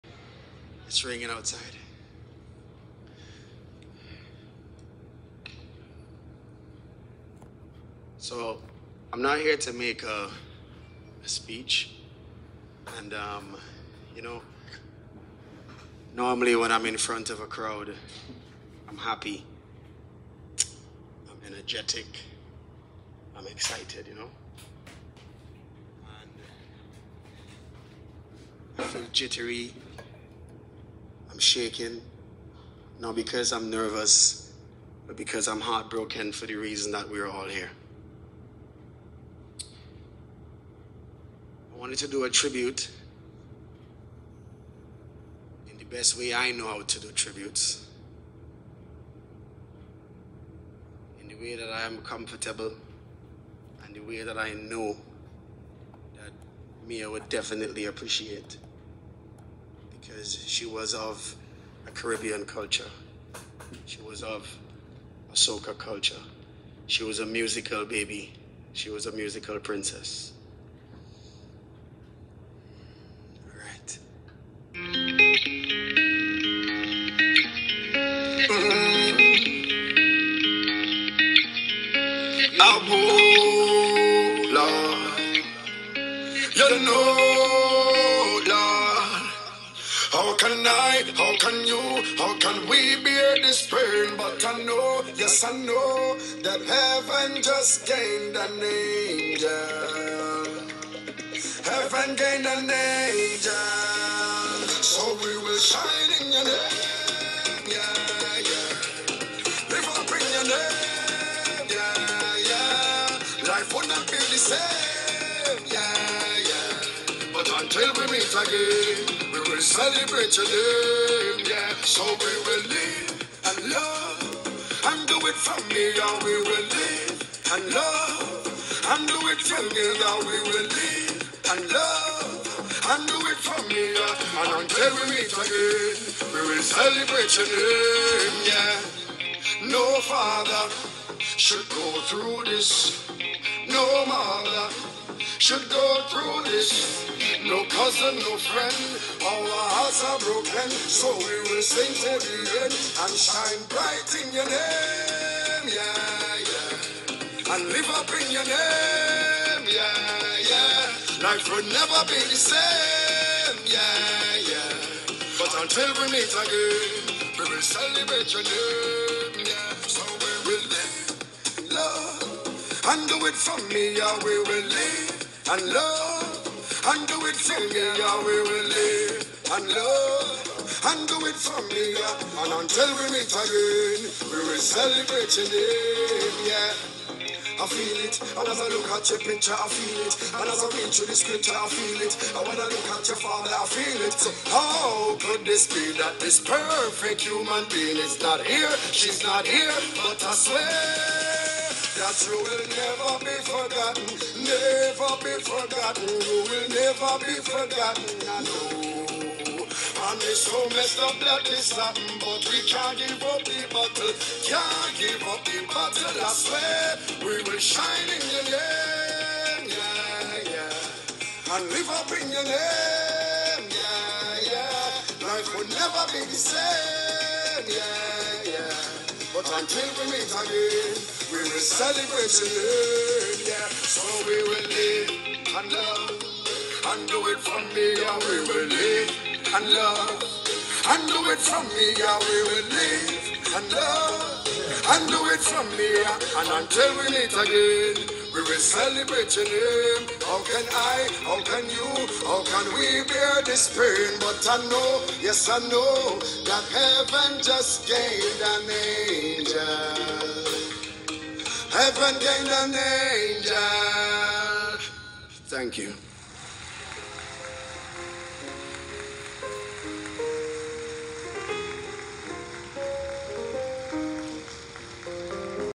Skinny Fabulous wrote and sang his love for MiyaMacano
Service and Celebration of Life for Miya Marcano